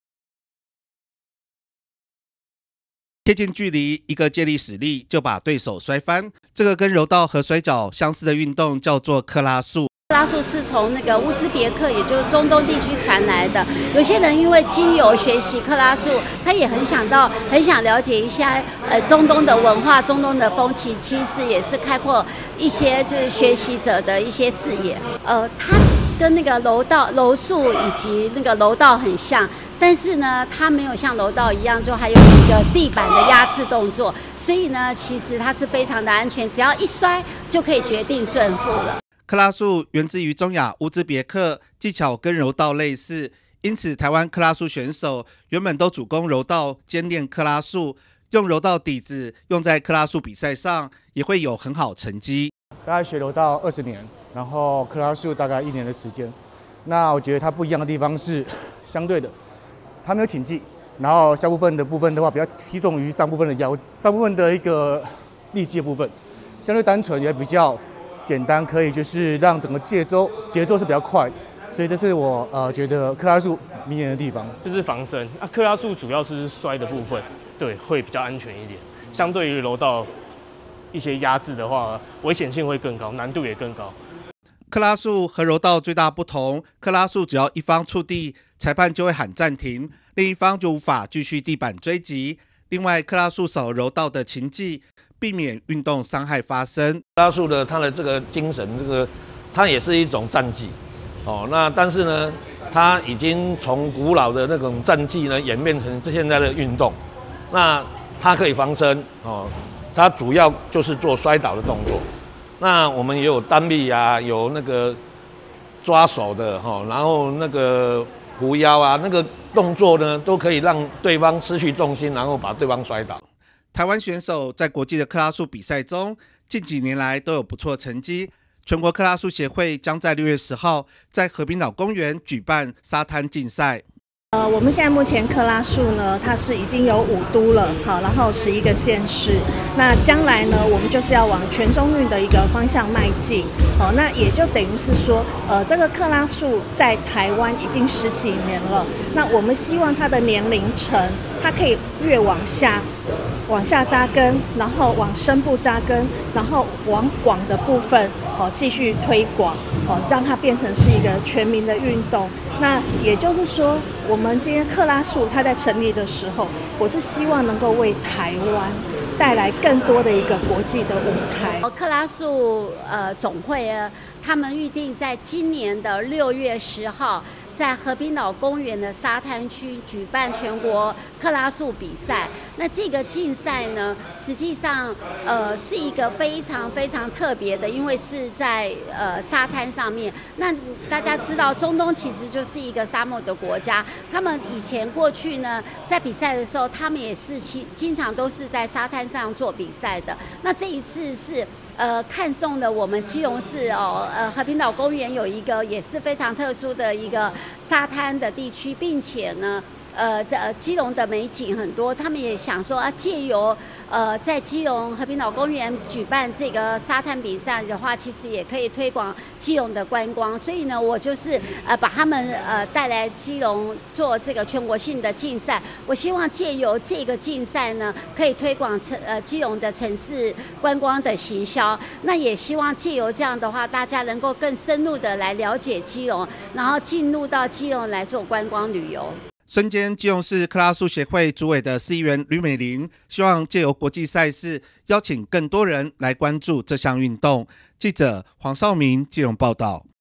烏茲別克|克拉術|烏茲別克傳統武術 "克拉術"減少運動傷害
(中嘉吉隆新聞 記者黃紹銘/基隆報導) 2023年克拉術亞洲錦標賽4月在中國杭州登場，這次台灣代表隊獲得1金1銀4銅佳績，為推廣這項跟柔道、摔角很像的克拉術，中華民國克拉術協會預計6月10日將在基隆和平島舉辦全國賽事，身兼基隆市克拉術委員會主委的市議員呂美玲，大力推廣賽事之外，也希望藉由大型比賽，讓更人認識基隆。
比賽前兩位選手先禮後兵，展開競技。
貼近距離，一個借力使力，就把對手摔翻，這個跟柔道與摔角的運動，叫做克拉術。
克拉術源自於中亞烏茲別克，技巧與柔道類似，因此，台灣克拉術選手，原本都是主攻柔道兼練克拉術，用柔道底子，在克拉術比賽上，也會有很好的成績。
克拉術與柔道最大不同，克拉術只要一方觸地，裁判就會喊暫停，另一方無法繼續地板追擊，另外，克拉術少了柔道的擒技，避免運動傷害發生。
台灣選手在國際克拉術賽事中，近幾年來都有不錯成績，全國克拉術協會將在6/10在和平島公園舉辦。
競賽，身兼基隆市克拉術委員會主委 呂美玲，希望藉由全國賽事，邀請更多人來關注這項運動。